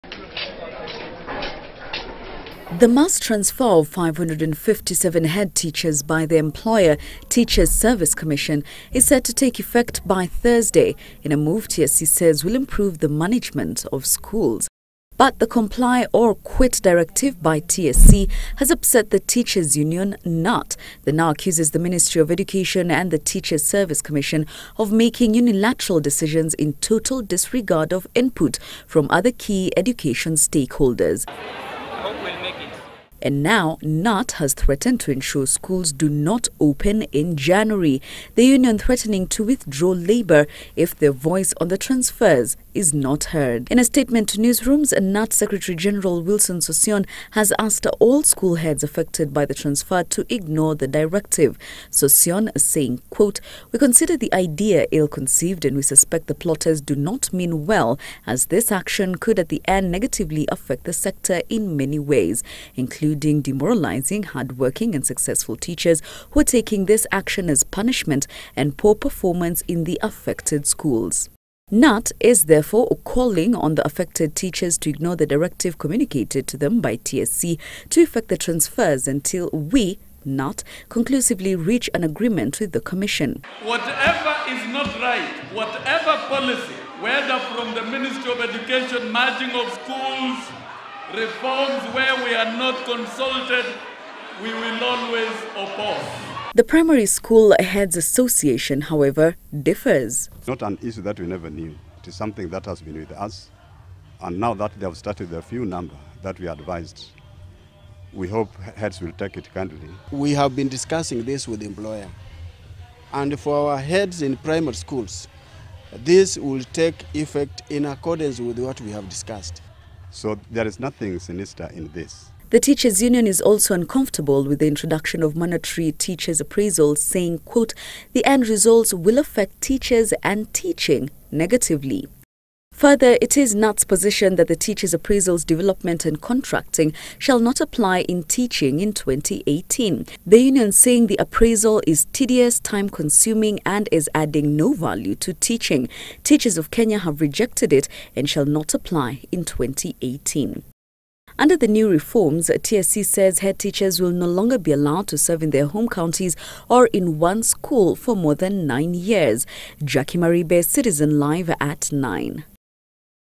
Knut cries foul over transfer of head teachers,Union threatens to call strike next week
Knut cries foul over transfer of head teachers

Union threatens to call strike next week

Knut says transfers unfair to hard working teachers

TSC has transferred 557 head teachers

Move aimed at improving management of schools

Primary school heads back transfers